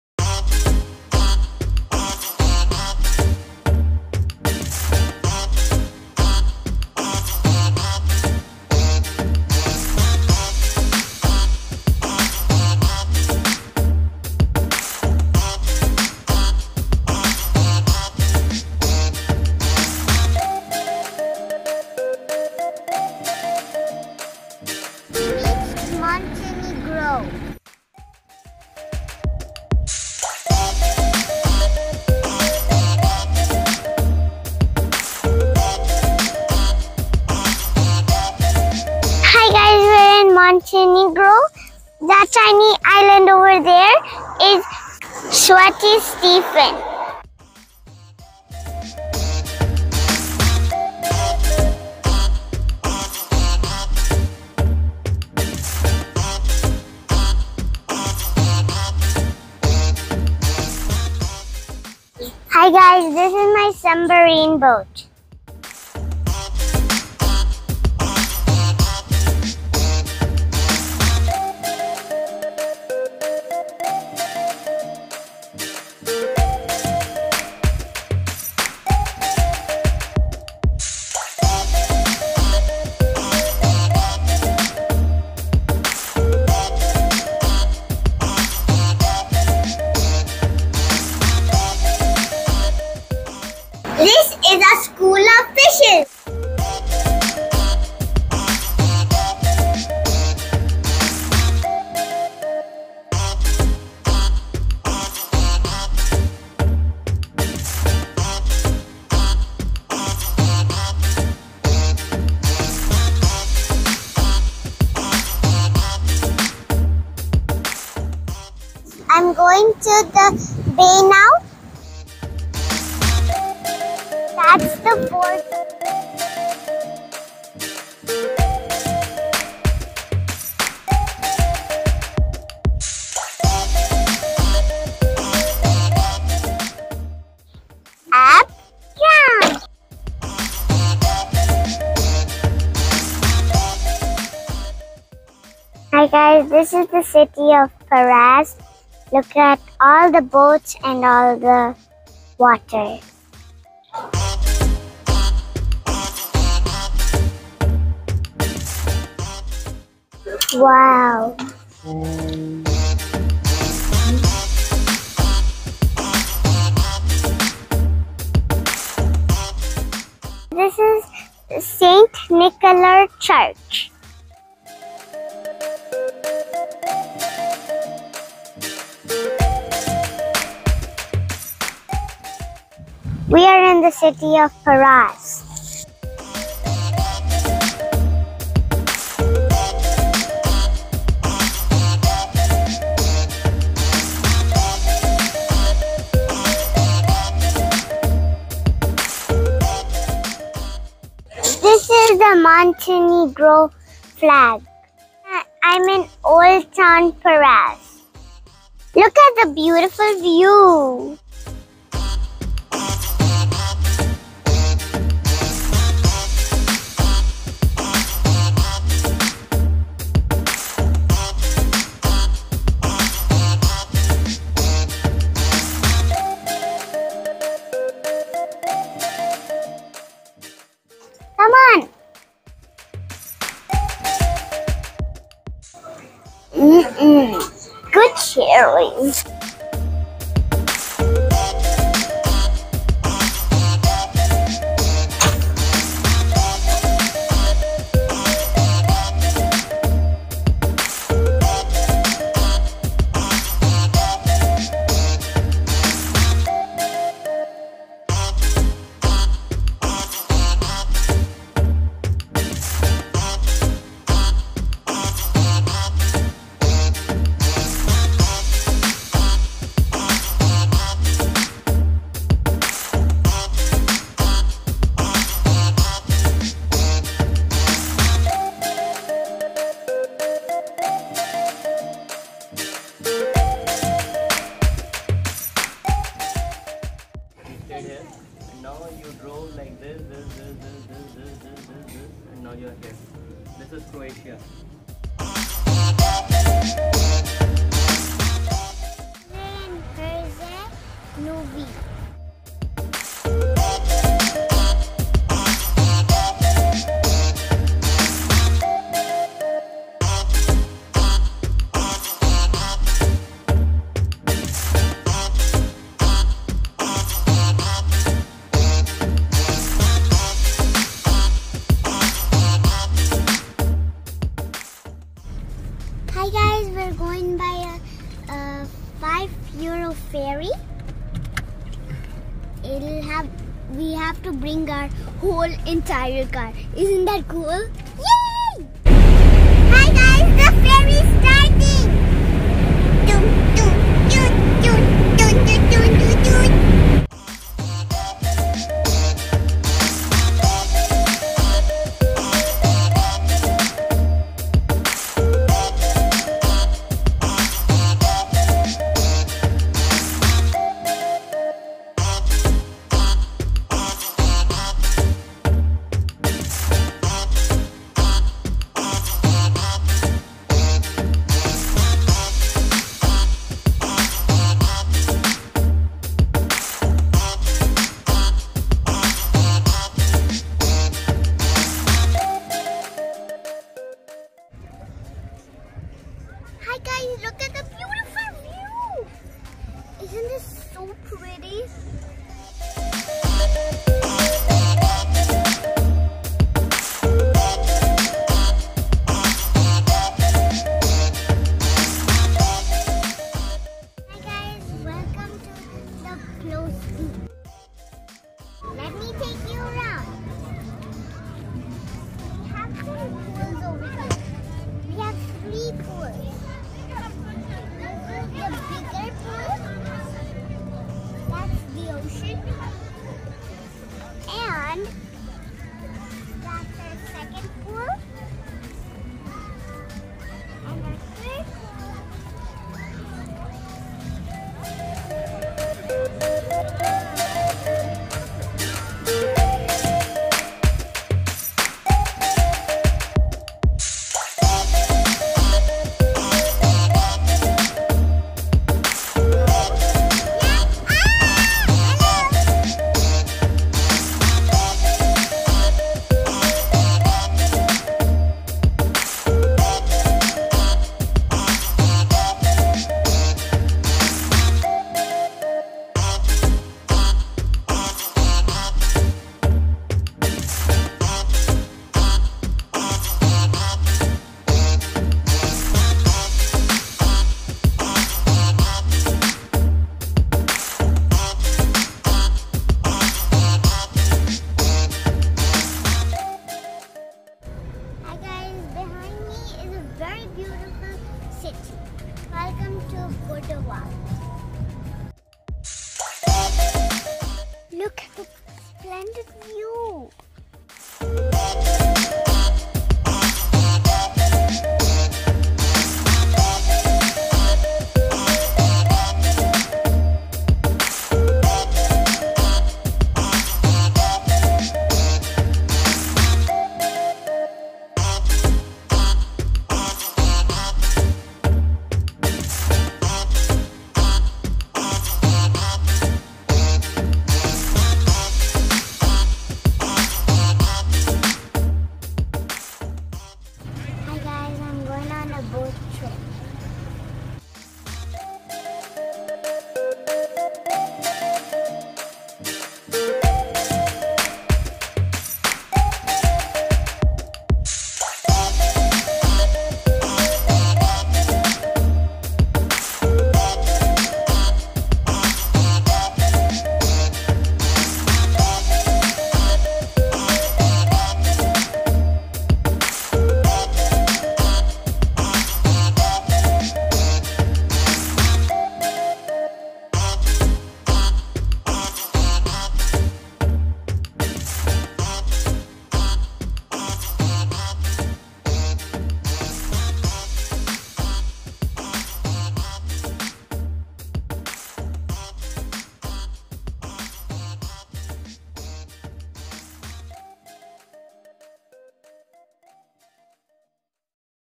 Travel to Montenegro with Kids | Road travel
and then Press the 🔔 icon to get all the latest travel updates and hotel reviews.

We got Montenegro Visa at Albania border FREE of charge with USA tourist  visa  very easily and then I travelled from Albania to Montenegro by Rental Car ,Met many lovely people and stayed in iconic Hostel in Kotor Old Town.